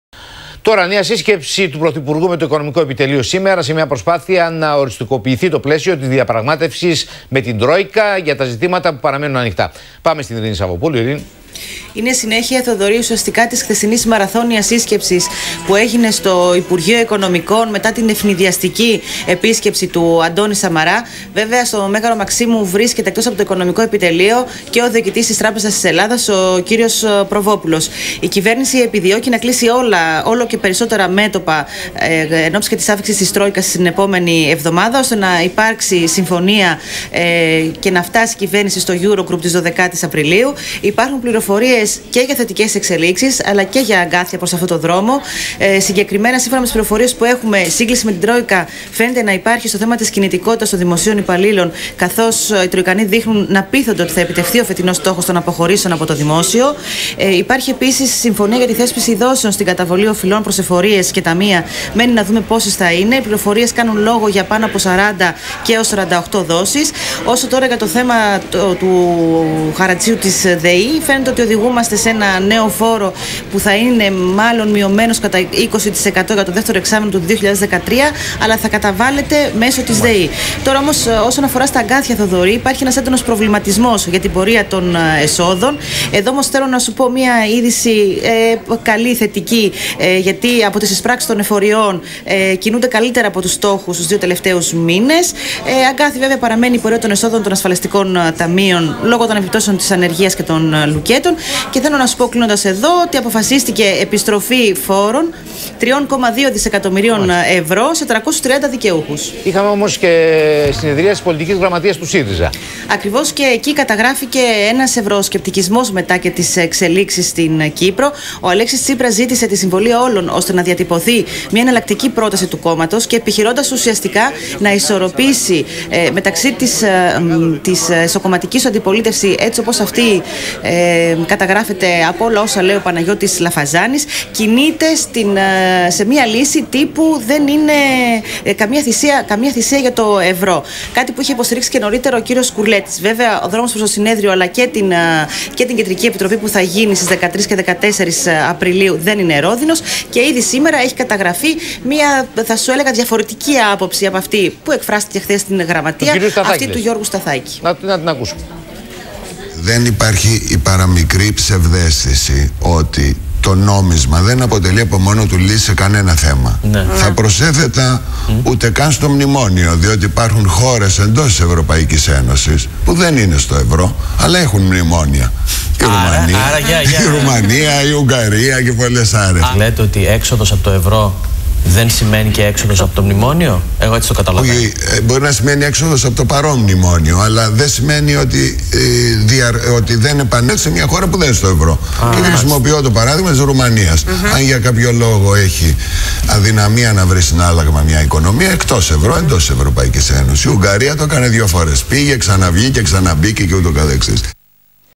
29.3.13-Νέα σύσκεψη Α.Σαμαρά με το οικονομικό επιτελείο.
Μεσημεριανό δελτίο ειδήσεων - Παρασκευή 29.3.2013 - Με τον Θοδωρή Δρακάκη - Νέα σύσκεψη Α.Σαμαρά με το οικονομικό επιτελείο. Γ.Σταθάκης: Έξοδος από το ευρώ δεν σημαίνει έξοδος από το μνημόνιο.